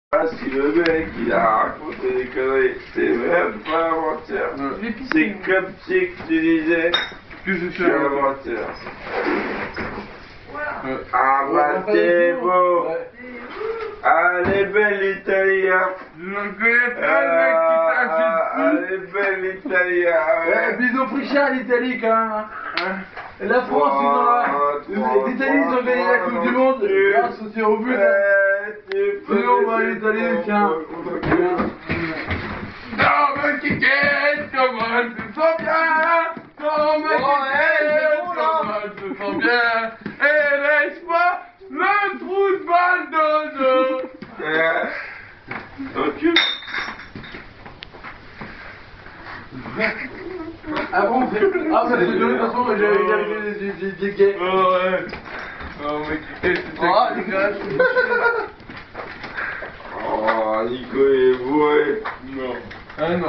Kweet déchiré
Une soiré bien arrosé, qui se finis en cul!